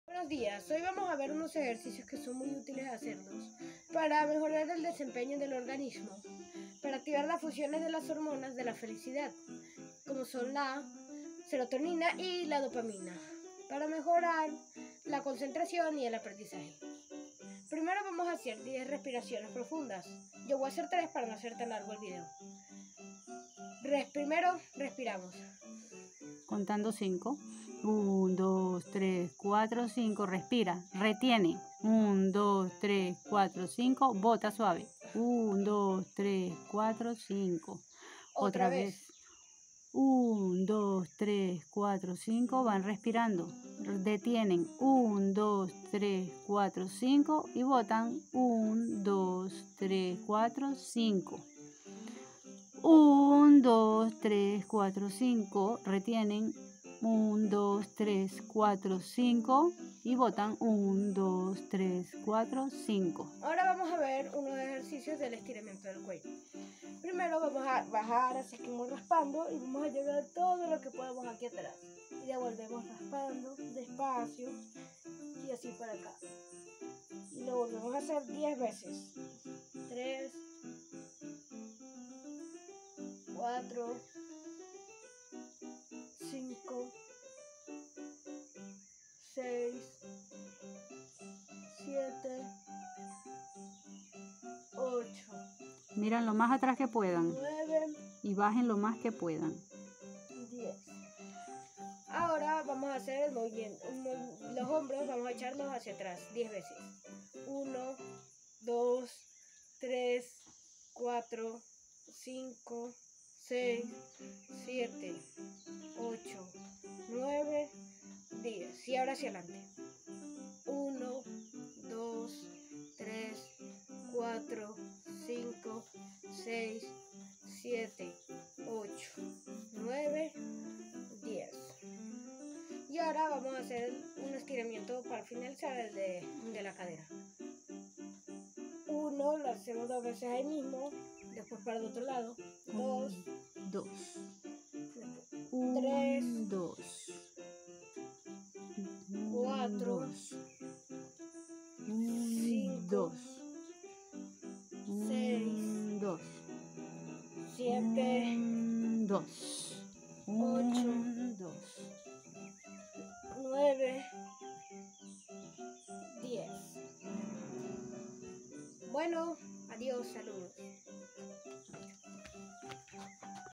Ejercicios para mejorar tu memoria y aprendizaje. Quita la ansiedad y el estrés.
Para aprender más rápido,  concentrarte, relajarte y hasta para ayudar a tu salud en general. sigue estos muy fáciles consejos y verás como tú concentración y salud física se restaura increíblemente.